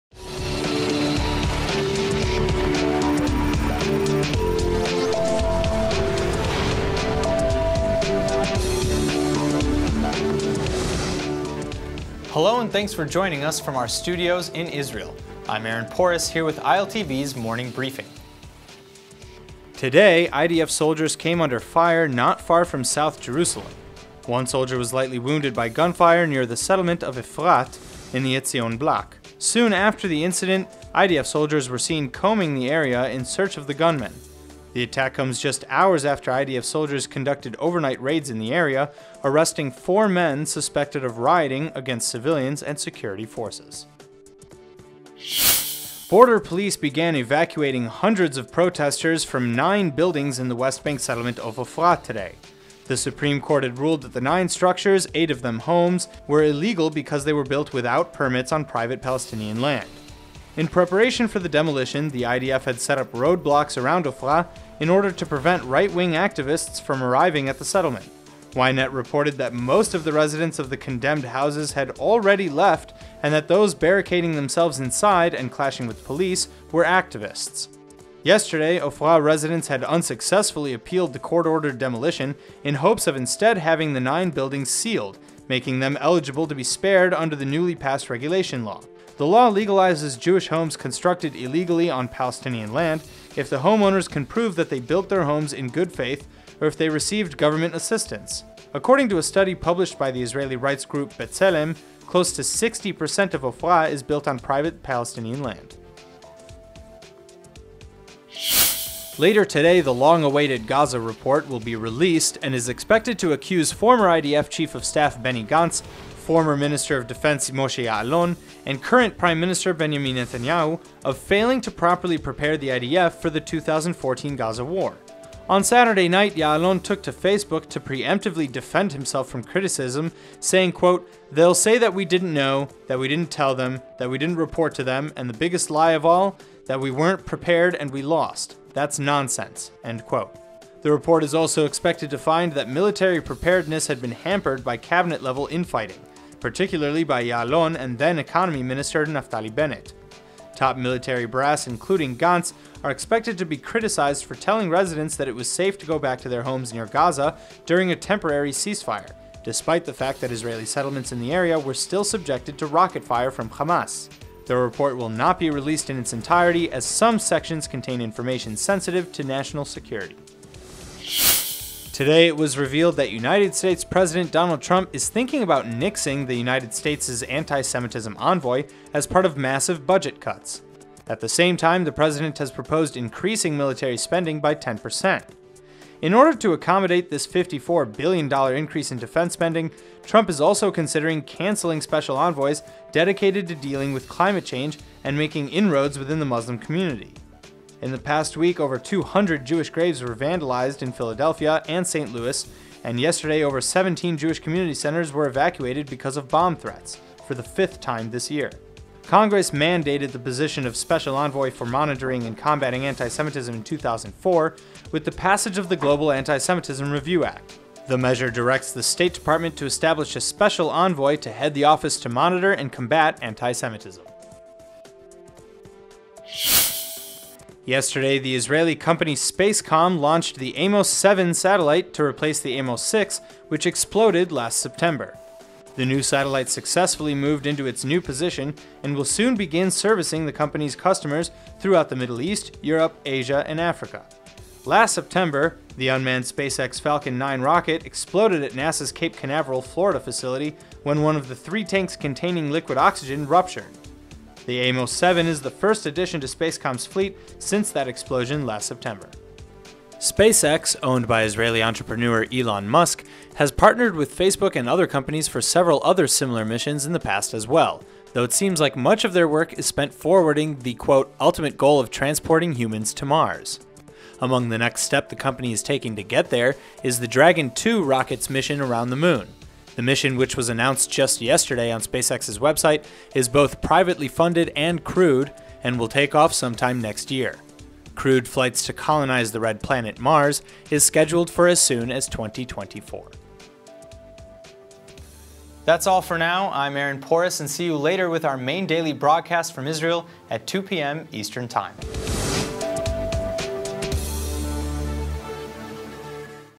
Your Morning News From Israel - February 28, 2017.
IDF soldier lightly wounded by gunfire, police clash protesters in Ofra, Gaza war report to be released later today, Trump considers nixing special envoy against anti-semitism, and Israeli company launches new satellite.